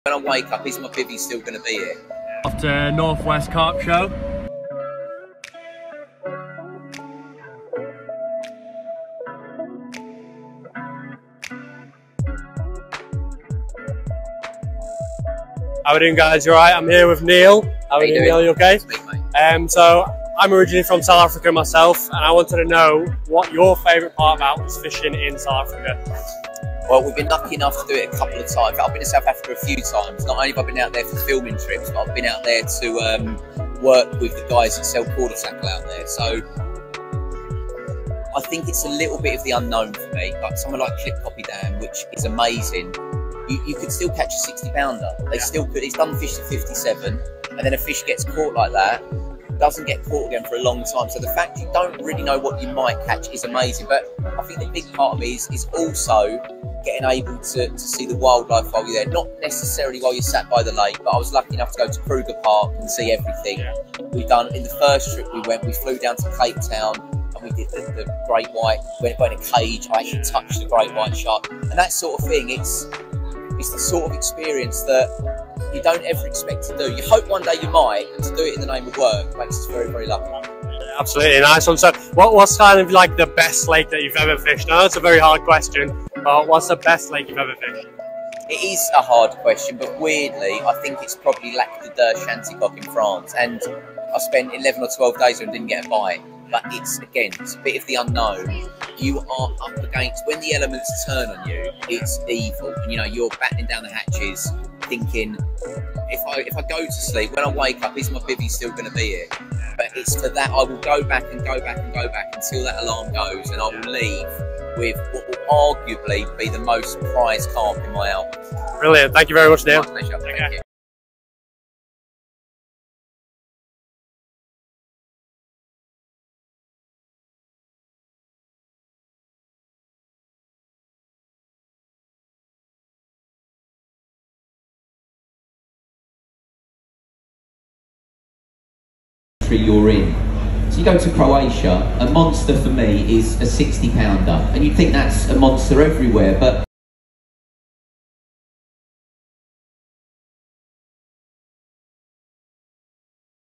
NORTH WEST CARP SHOW - WITH NEIL SPOONER MONSTER CARP SOUTH AFRICA - TOM HILTON
Headed to the North West Carp Show to see all the incredible stands, tackle brands and bait companies! We also bumped into Neil Spooner and had a chat about South Africa when they were filming Monster Carp! Also his favourite lake!!! Check it out!